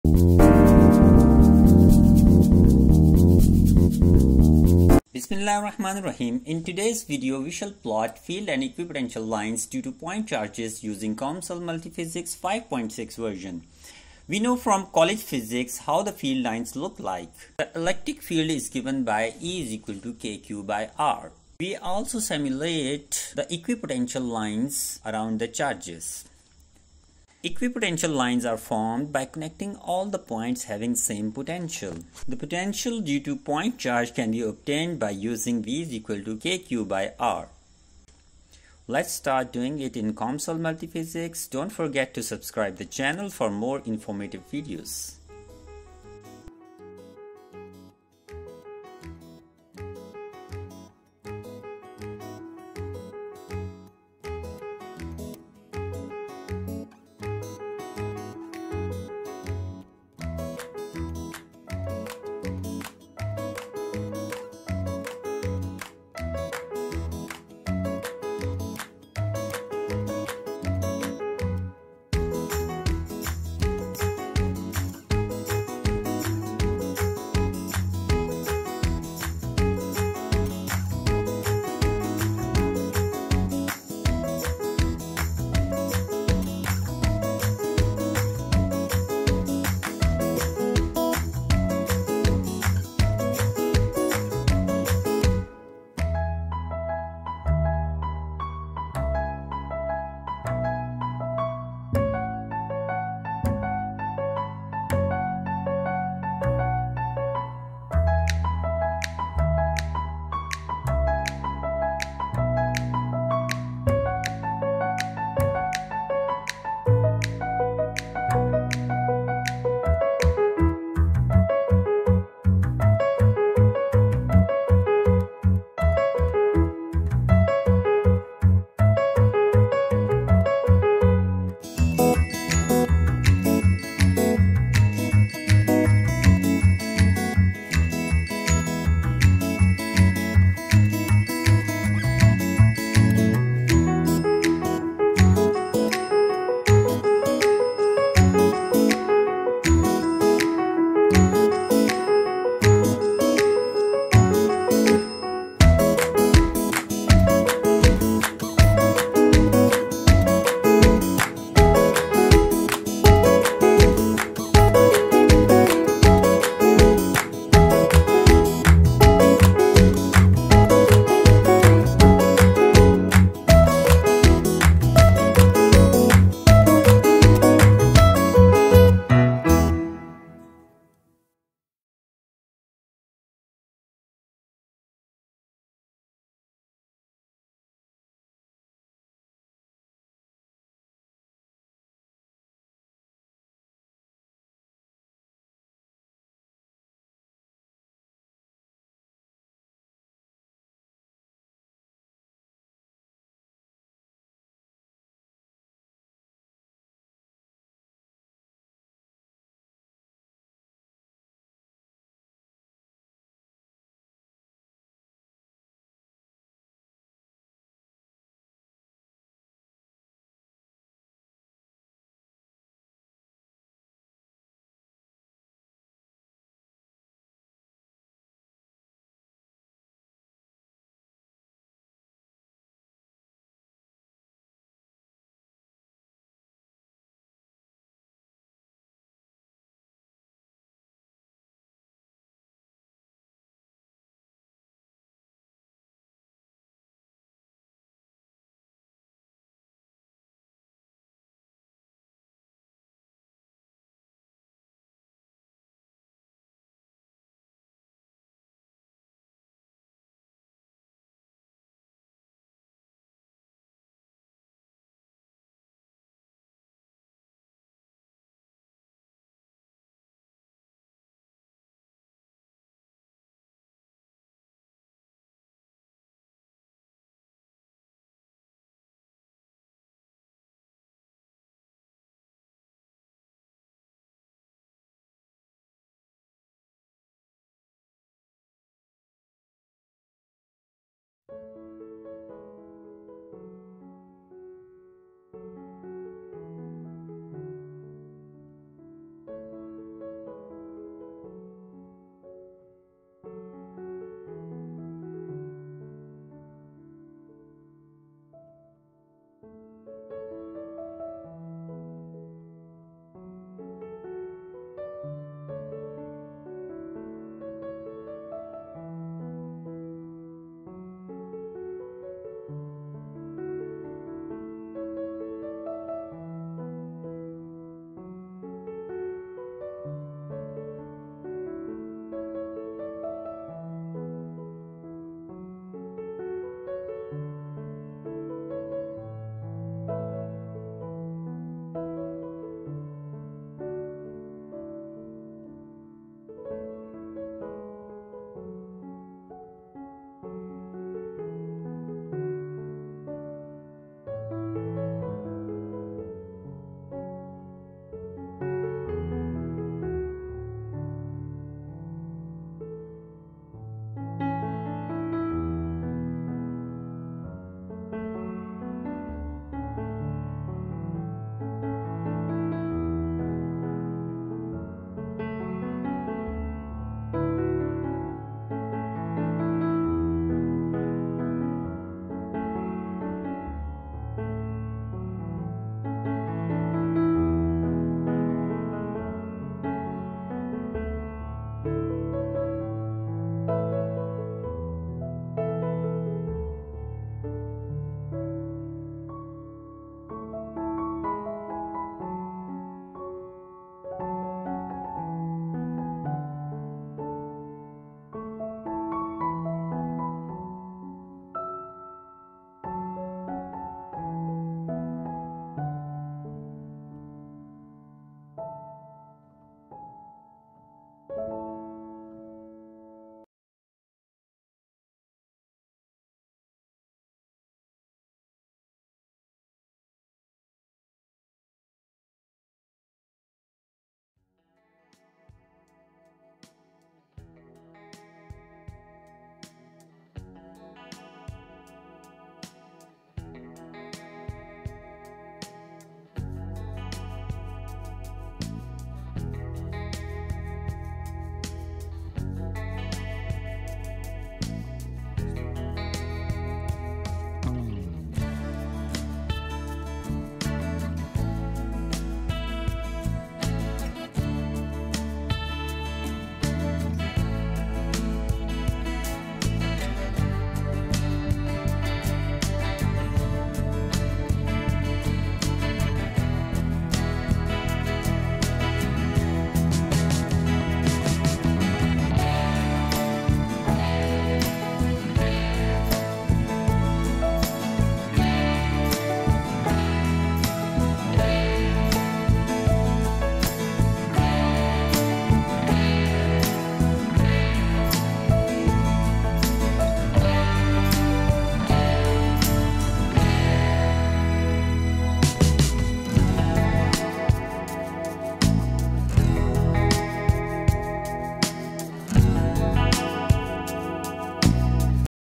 Field and equipotential lines due to point charges using COMSOL 5.6
Basic use electrostatic module of COMSOL is explained.
Thanks and Regards
M.Qasim Khan